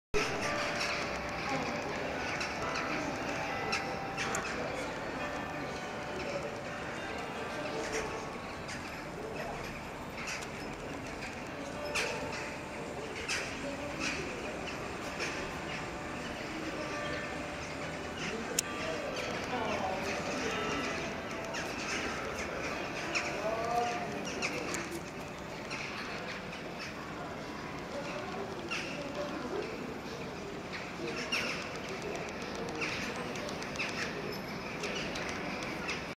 Luxembirds! Crow Gathering, Luxembourg
So you wish to bear witness to the full-moon Halloween night Great Gathering of the Crows of 2011.  Be warned!  I'd love to tell you what these are, but I've sworn a solemn oath never to reveal the secrets of the crows.  I've said too much already...